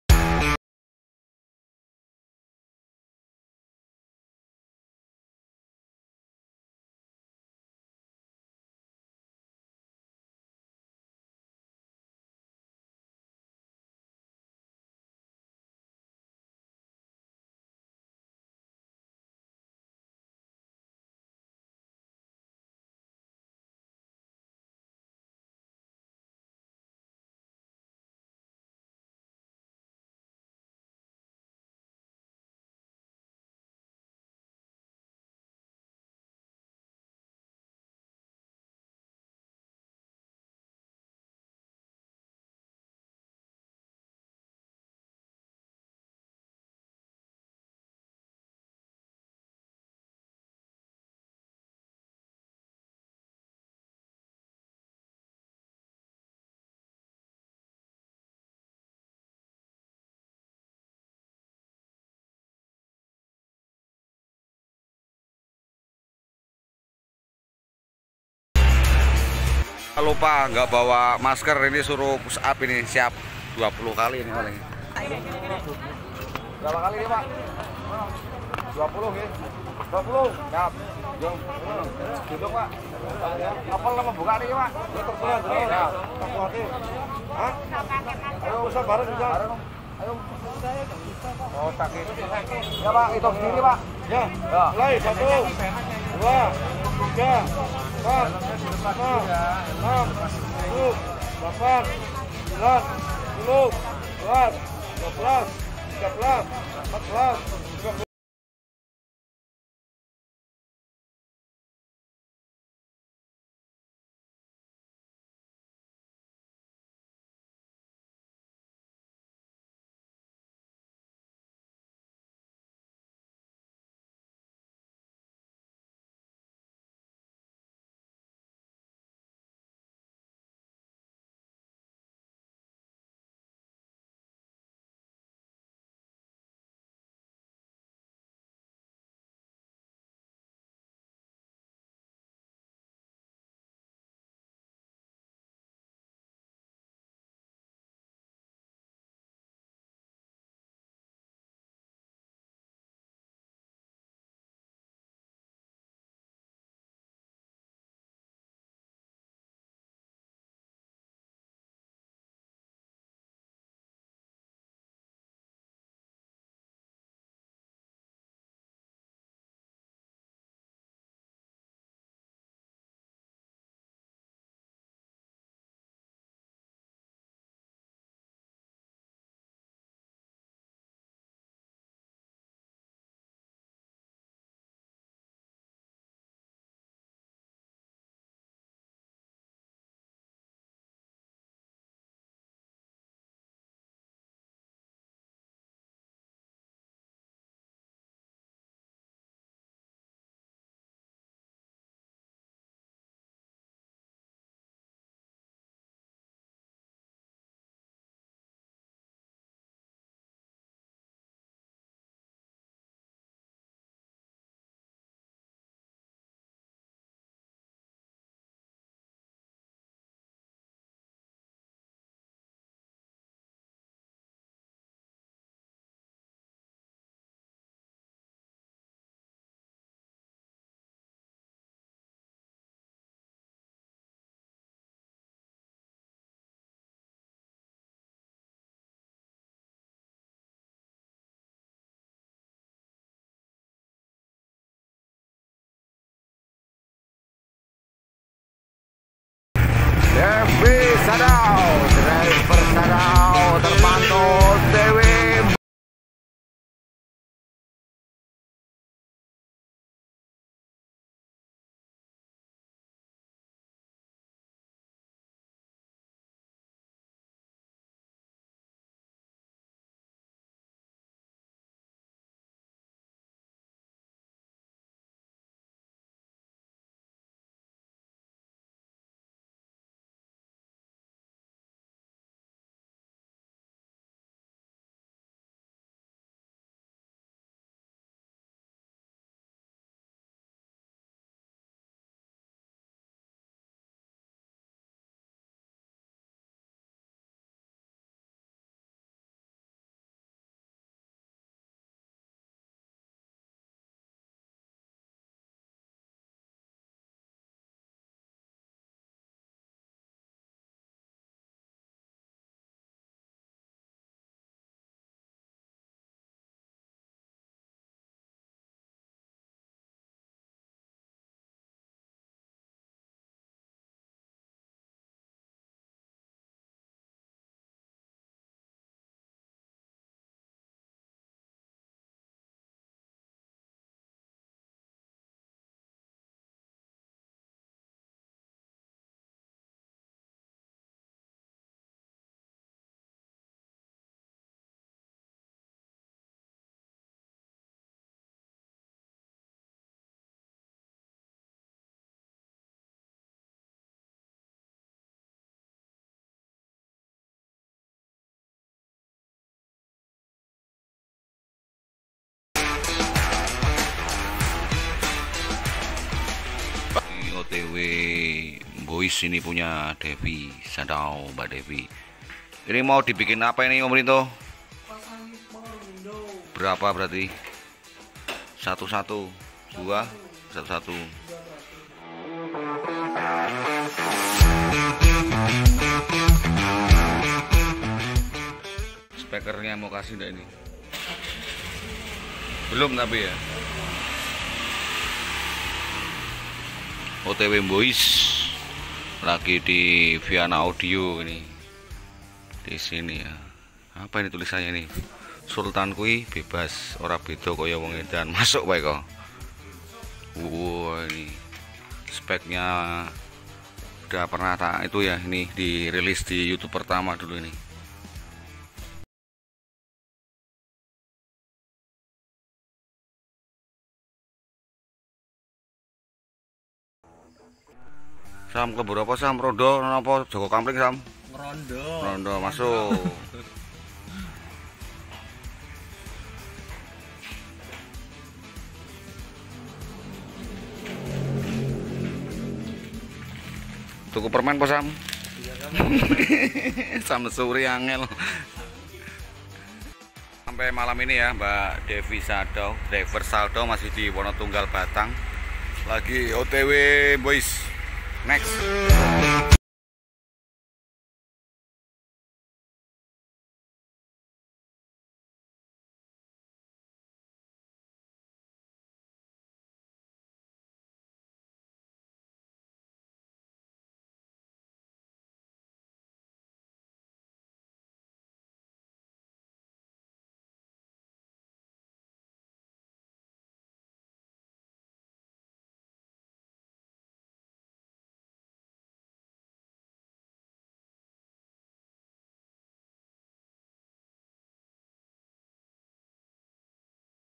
Kena Oprasi !! Saat kawal wahyu Abadi dan Devi Shadow17
pengawalan truck wahyu abadi dan dilanjut driver shadow yang mau pasang variasi di viana Audio wonotunggal Batang